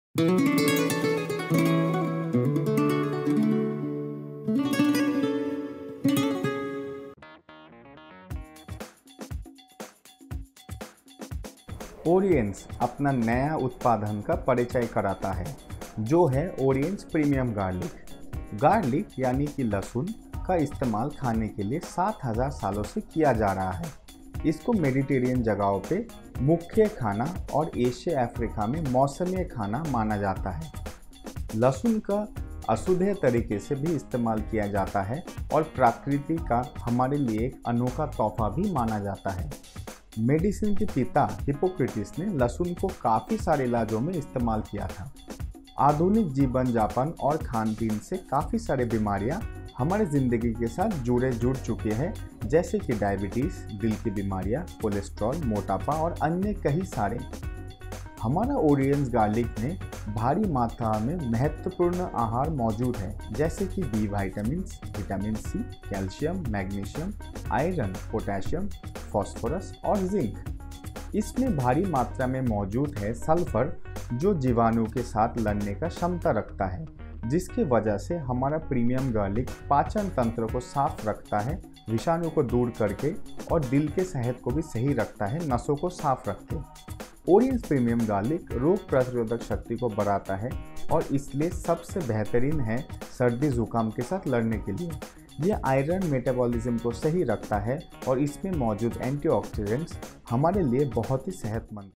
Premium Garlic Tablets Available Online | Improves Metabolism | Oriens | Helps Digestion | Hindi
Premium Garlic - Nature’s Best Support for Heart.
Orien’s Premium Garlic is a quality health supplement for various biological functions.

Key Ingredients:
Garlic extract (Allium sativum)
Allicin is the major biologically active component of garlic. Key the ingredient responsible for the broad spectrum of anti-bacterial and cardioprotective activity of garlic.

Health Benefits:
          • Helps to maintain a healthy heart.
          • Helps to reduce urinary tract infections.
          • Helps to clear the bronchial tract and alveoli.
          • Helps to Improve iron metabolism.
          • Carminative & helps in digestion.
          • Helps to prevent respiratory infections.

Why Oriens Premium Garlic is the best?
1. Stimulate and boost the immune system.
2. Blood-thinning capacity and cardioprotective effect.
3. Is Rich in antioxidant property

Address: FC-3, 9th Street, Ambattur Industrial Estate, South, Ambattur, Chennai, Tamil Nadu 600058